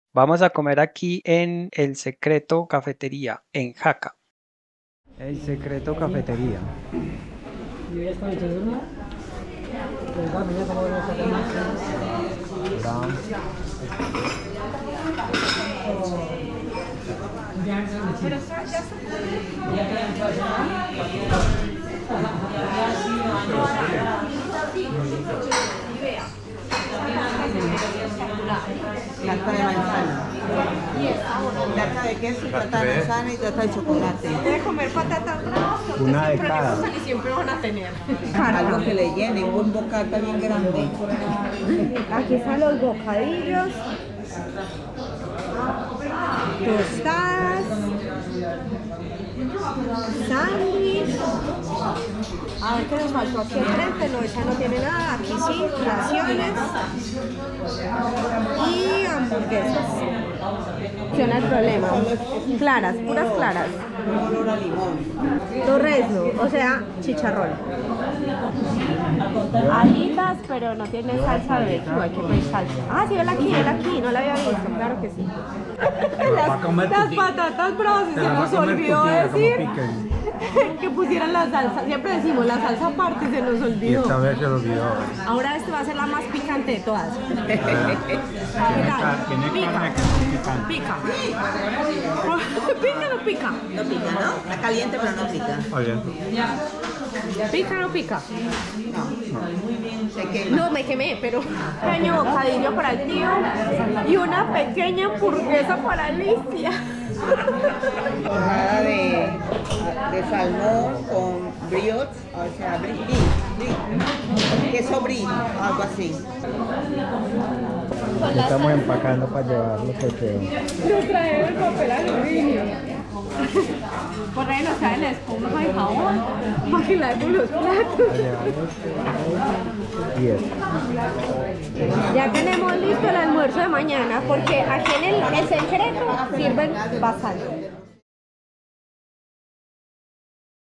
El SECRETO de la CAFETERIA El Secreto de Jaca una comida DELICIOSA ABUNDANTE y a buen PRECIO
Deseas apoyar a Uva Madura ?